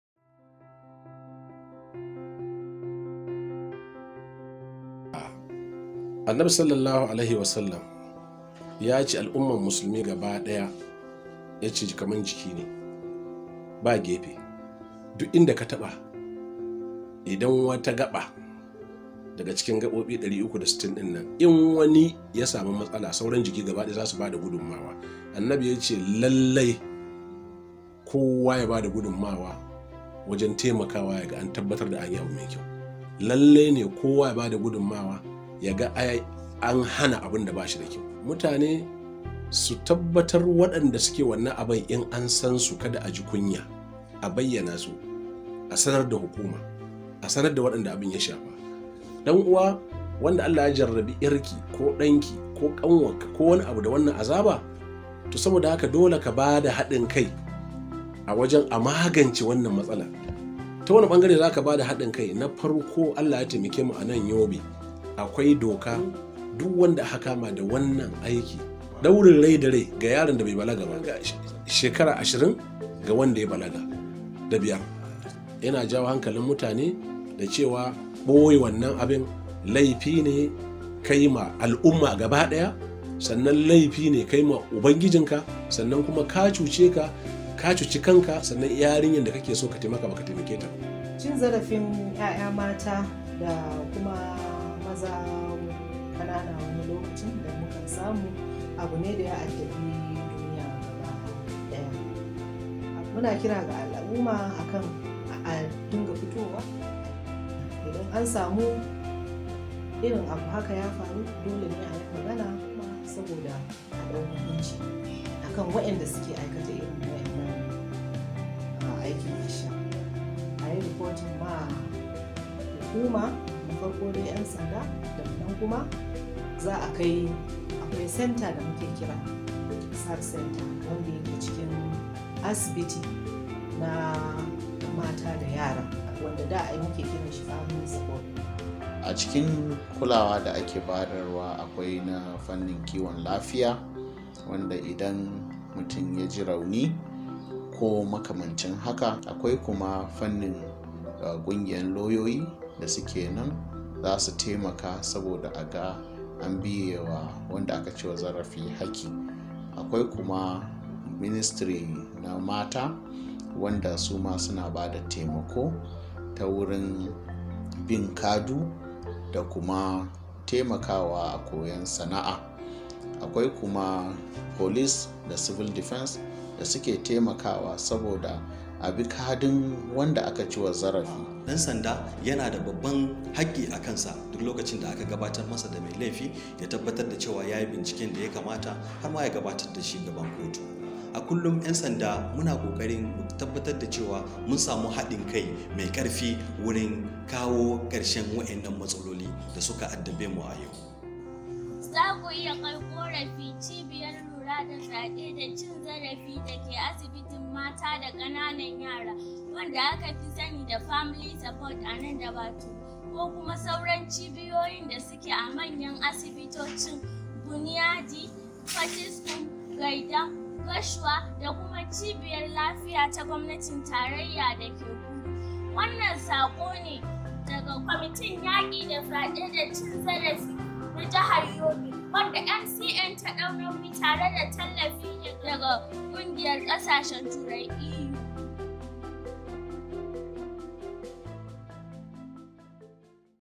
Response to Sexual and Gender Based Violence in Yobe State [Hausa version]
Manage by Managing Conflict in Northeast (MCN ) funded European Union and implemented by British Council